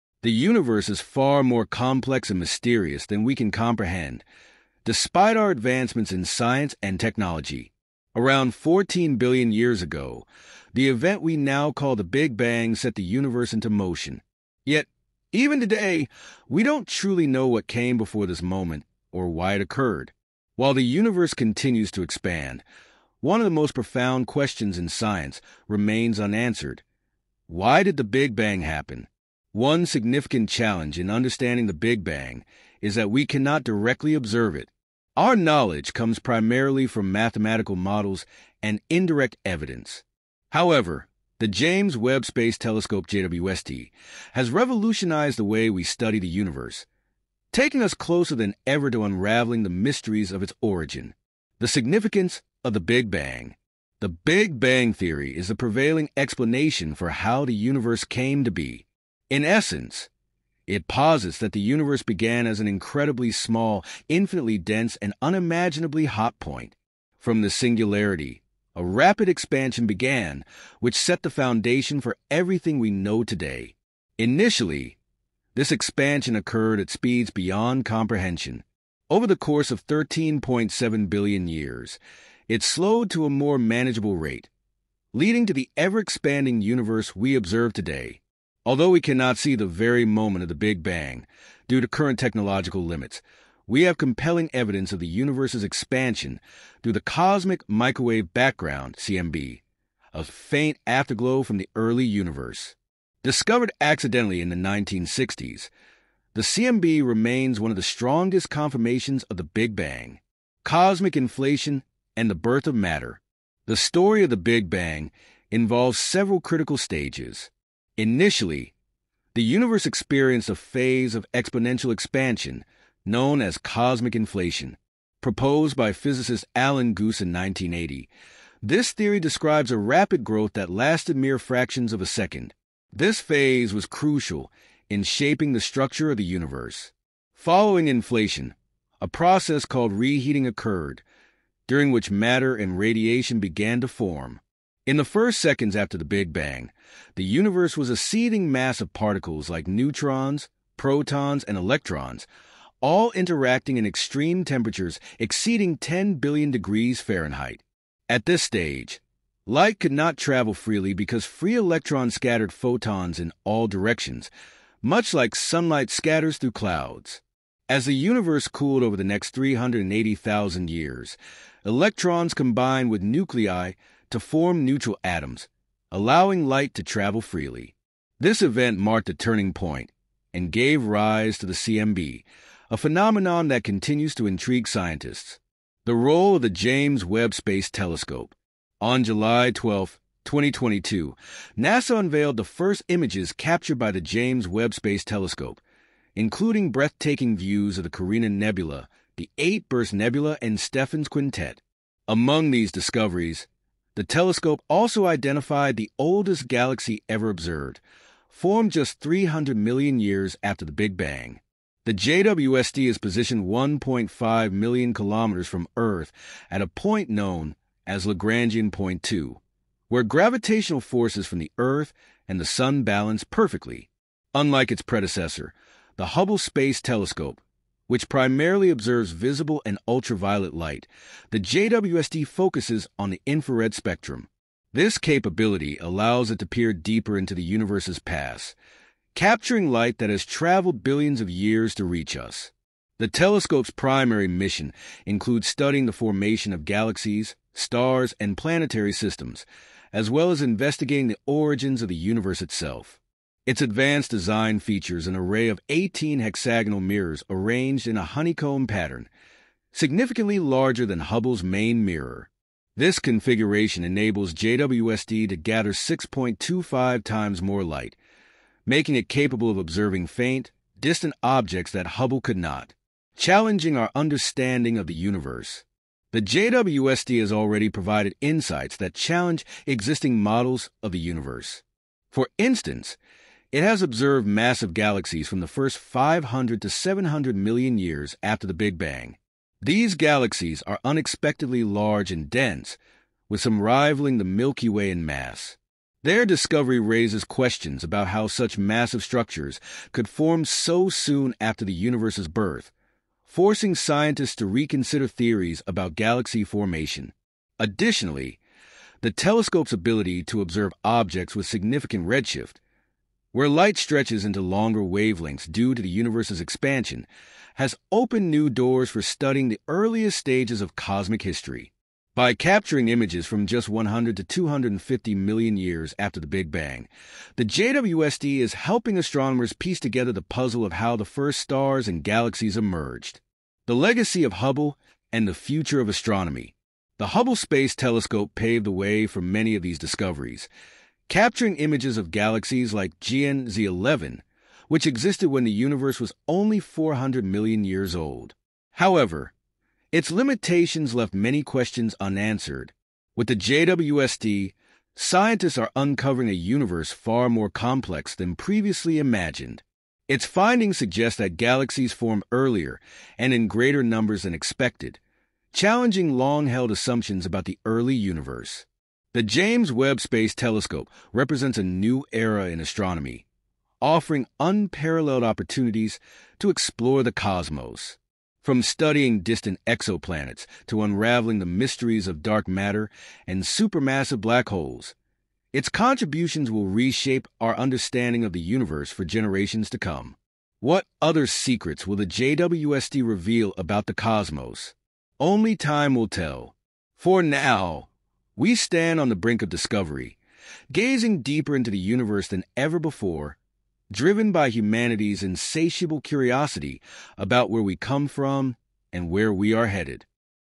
Breaking News: James Webb Unveils Discovery That Will Rewrite History
In this video, we explore the groundbreaking capabilities of the James Webb Space Telescope (JWST) and how it's revolutionizing our understanding of the universe. From taking the first pictures of the Big Bang to challenging existing theories of galaxy formation, JWST is shedding light on some of the most profound mysteries of space. Discover how this advanced technology is looking back in time, revealing the birth of stars, galaxies, and more, helping scientists get closer to answering the ultimate question: Why did the Big Bang happen? Join us as we dive into the future of astronomy and explore what JWST can reveal about our cosmos.